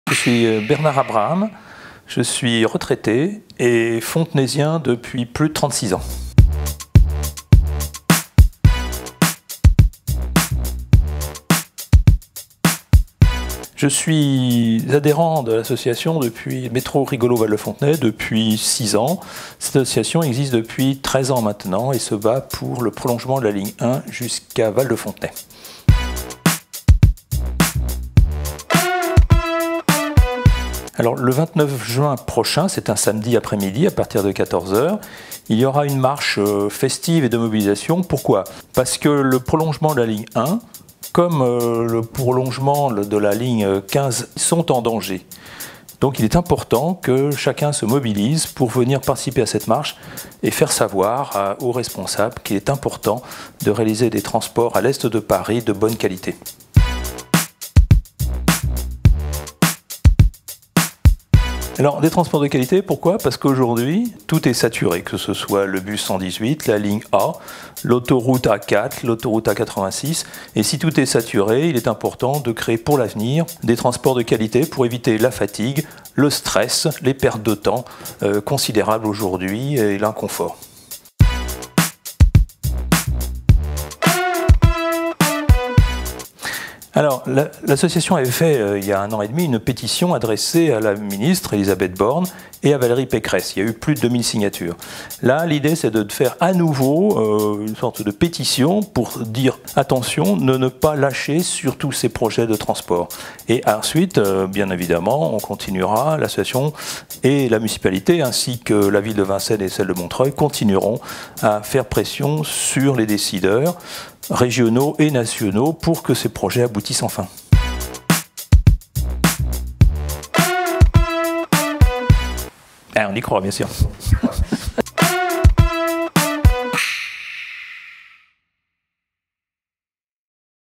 Marche pour le métro
Samedi, Fontenay marche pour le métro.

Parce que nous aussi, nous avons droit à des transports de qualité, l'association Métro Rigollots Val de Fontenay et la Municipalité organisent une mobilisation festive dans la ville

Pour l'occasion, FSB a rencontré Bernard Abraham, membre de l'association, qui nous explique pourquoi il est essentiel de se mobiliser samedi

Départ le samedi 29 juin à 14h de La halle Roublot